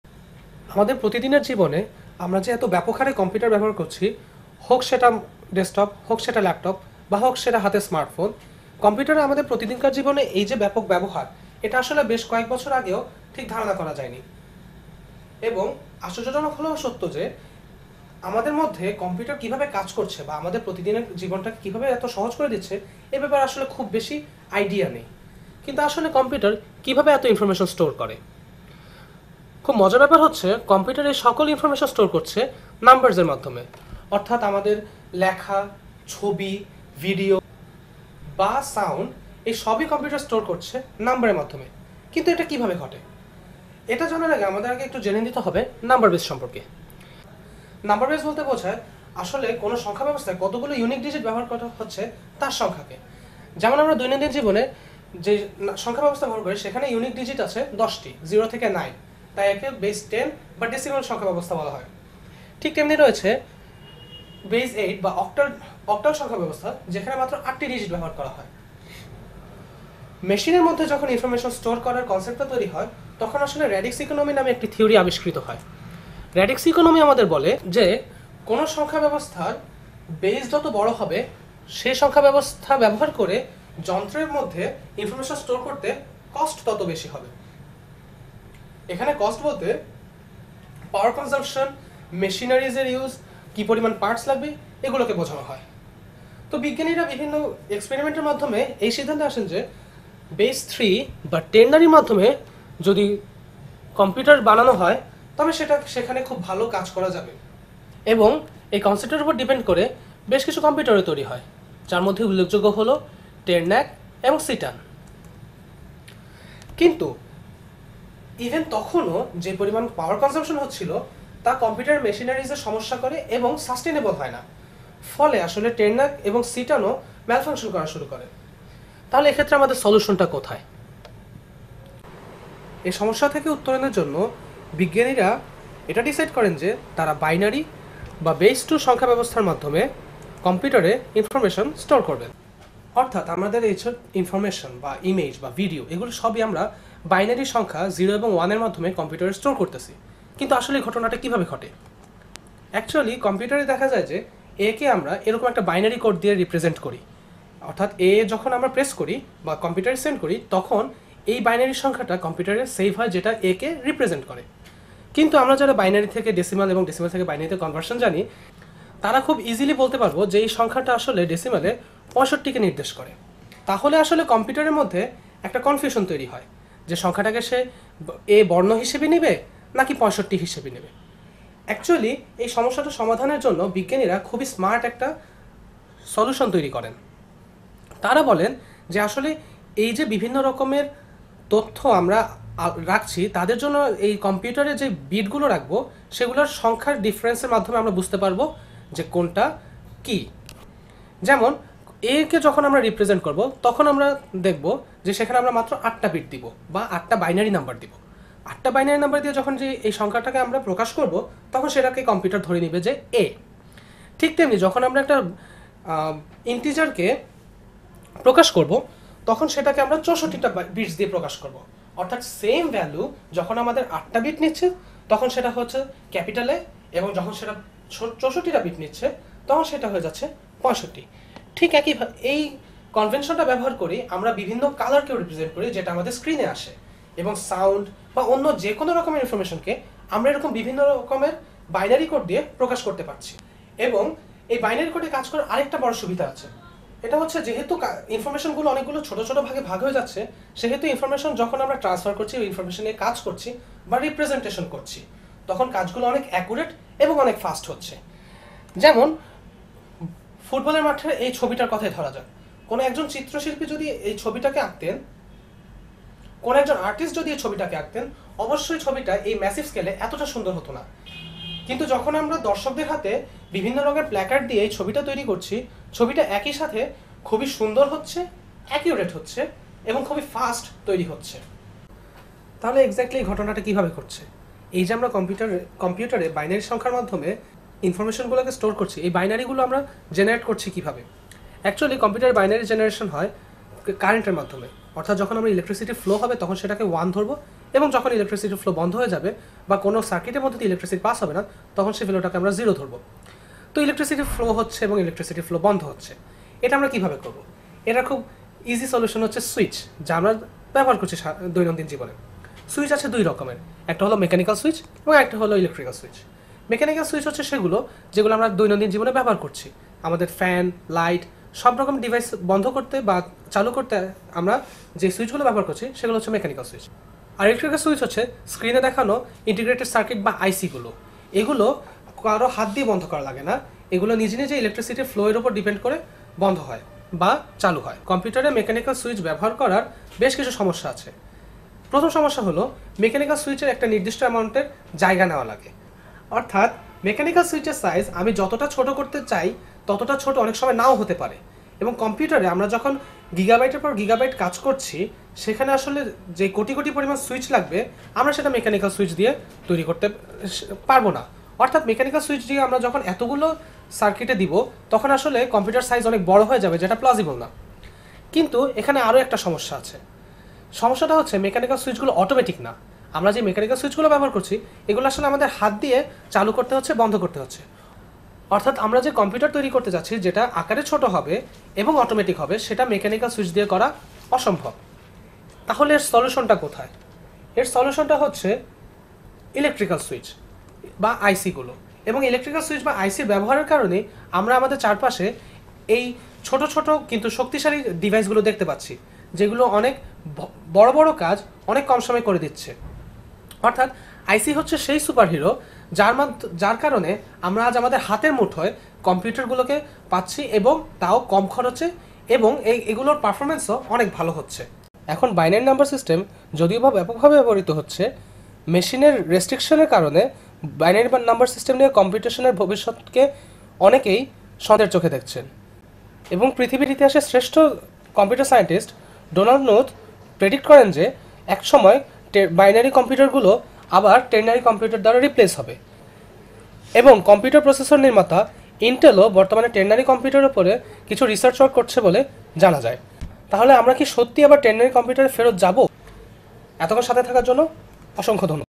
Binary in Computers - Why and How ?
This was made by some of the students of the Dept. of CSE, BUET as part of the EEE course targeting the Bangla speaking people.